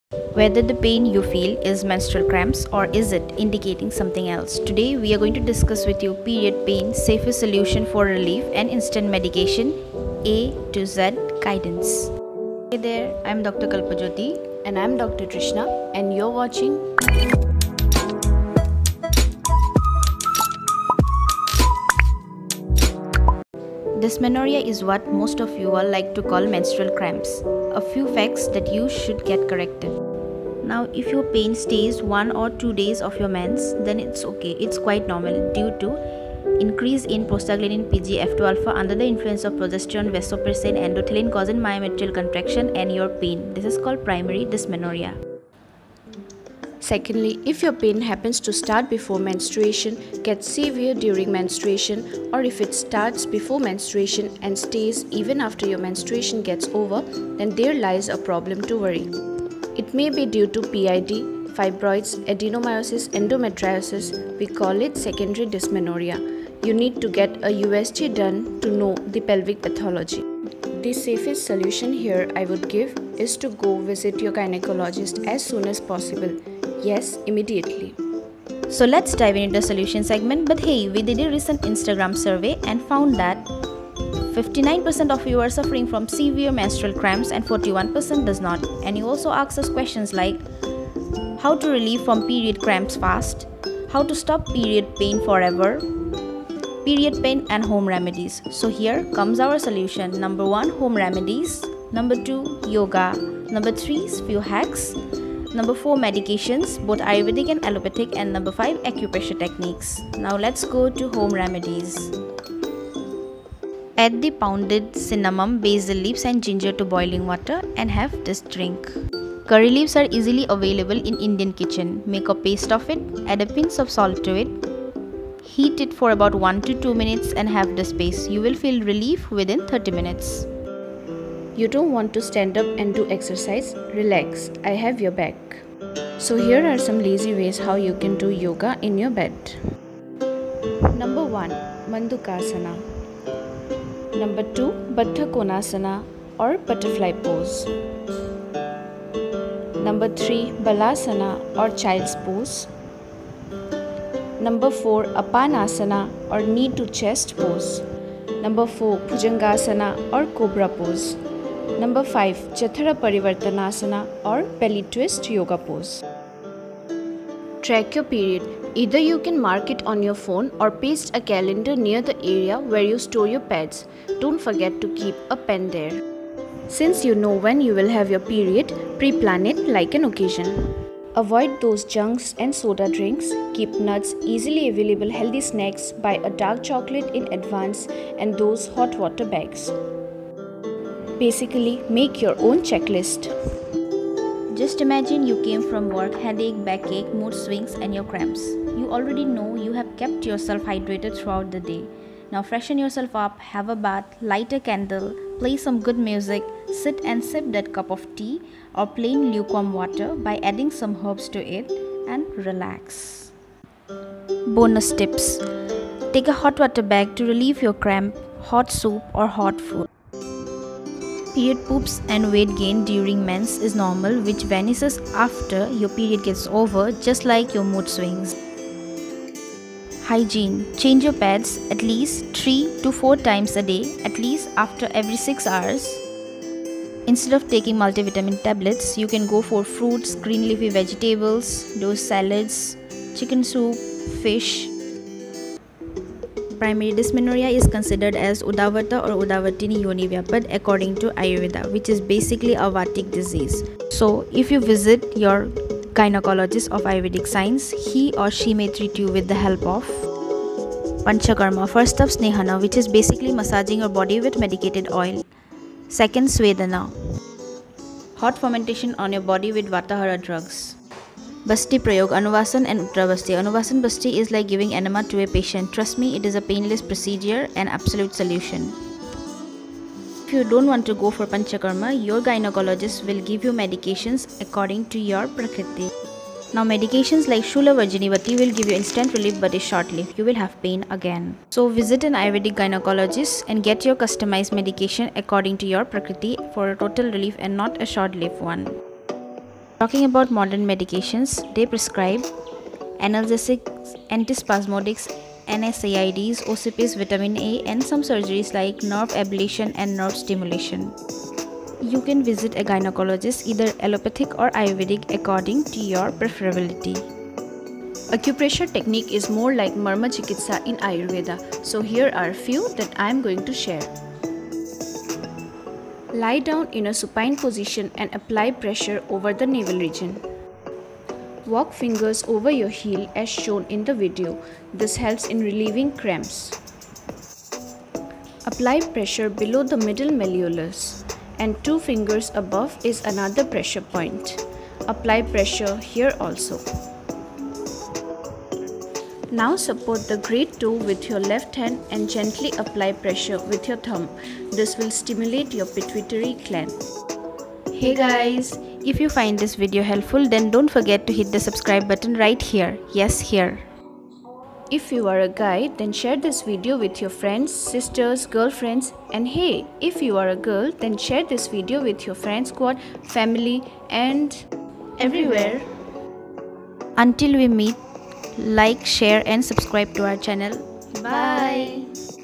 Period Pain |SAFE-INSTANT medication|( A-Z guidance) menstrualcramps besthomeremedies|Kalpajyoti
Do you have queries like,how to relieve period cramps fast? How to stop period pain forever? Period pain and home remedies...??
Relax,we have your back.
First question yourself , do you really suffer from menstrual cramps or is it something else?
We have given details about your period cramps and its safe, instant remedies,few yoga asanas and all essential stuffs.Hope its helfpul. Looking forward for your comments.
2.Instagram = whitecoatbees
Thanks and regards💜
White Coat Bees